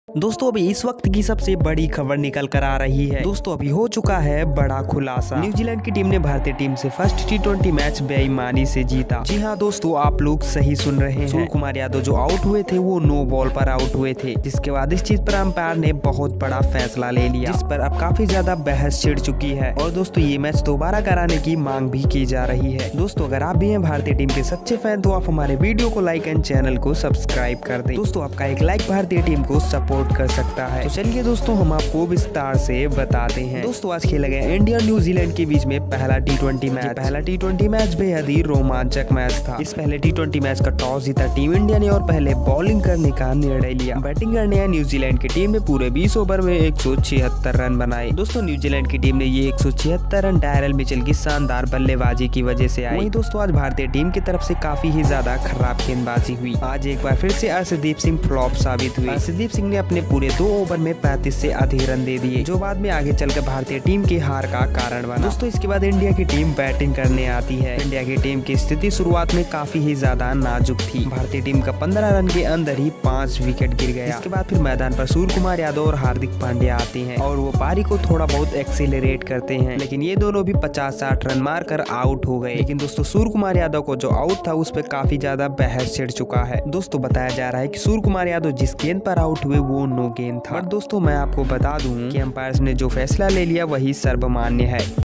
बड़ा खुलासा- न्यूजीलैंड ने भारत से पहला टी20 मैच बेईमानी से जीता
अभी इस वक्त बहुत बड़ा खुलसा हो गया है न्यूजीलैंड ने पहला टी20 मैच बेईमानी से जीता